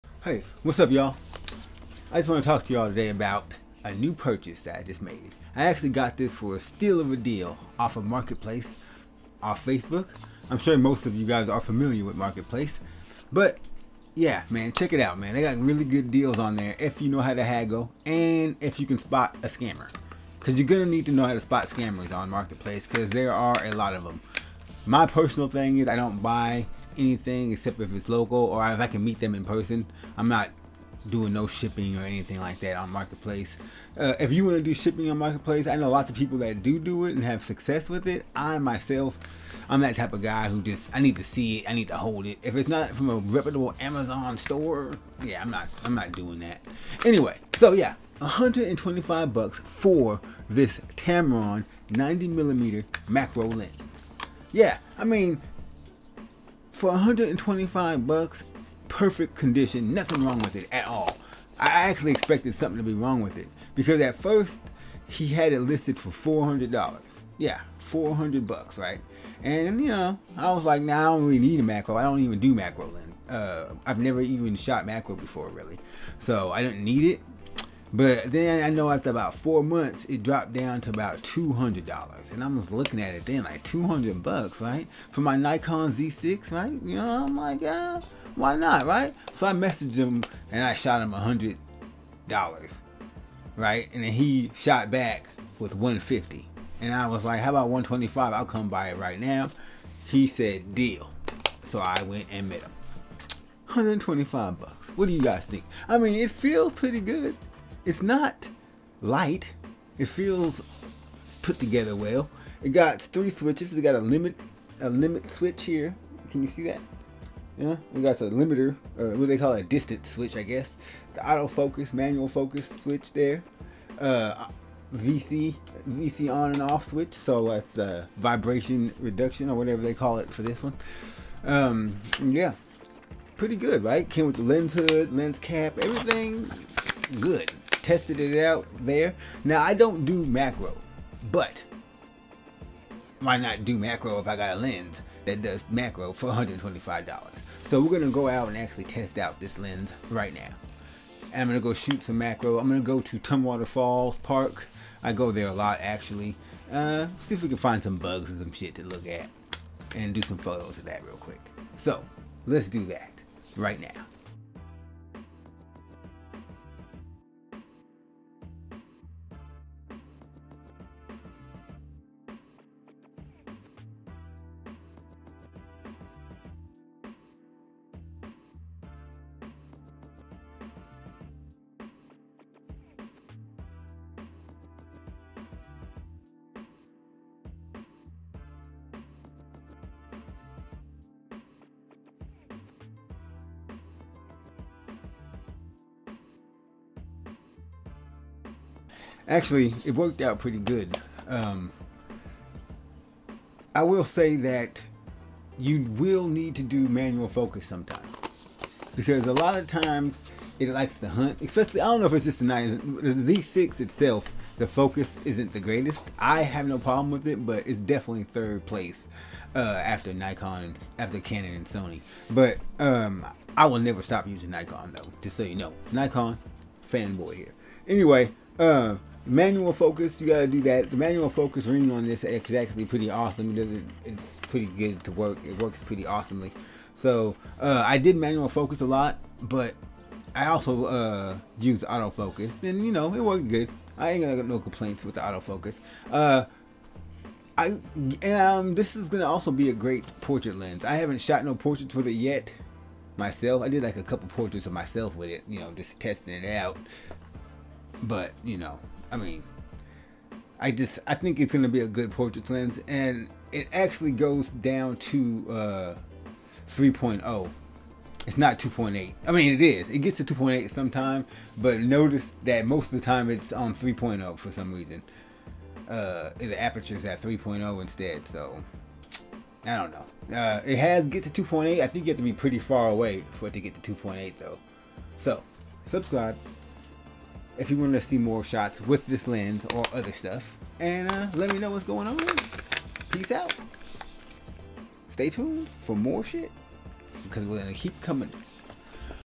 Nikon Z6 + Tamron 90mm VC Macro Lens: First Impressions and Photo Samples
In this video, I share my experience of using the Tamron 90mm VC macro lens on my Nikon Z6 camera. I bought this lens for a very low price on Facebook Marketplace and I was curious to see how it performs on the Z6. I tested the lens in different situations and took some sample photos to show you the results. I also talk about the pros and cons of this lens and whether I recommend it or not. If you are interested in macro photography and looking for a budget-friendly lens for your Nikon Z6, this video is for you.